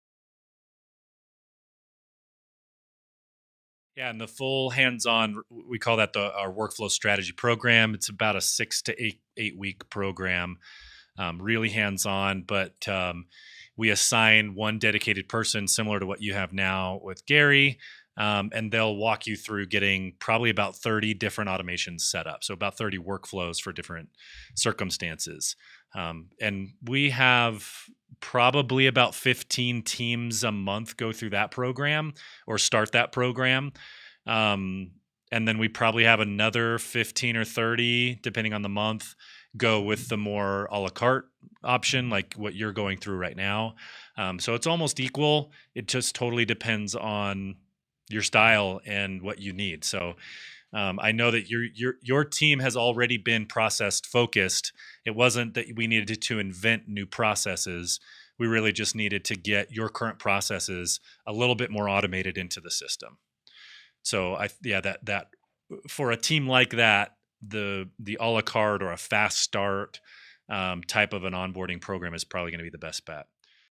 What Other Options are There to Ensure Maximum Benefit from Realvolve with Mark Deering
Mark Deering of the Deering Team in Grand Rapids, MI sat down with Realvolve to tell us how his recent transition from Contactually to Realvolve has gone for his team.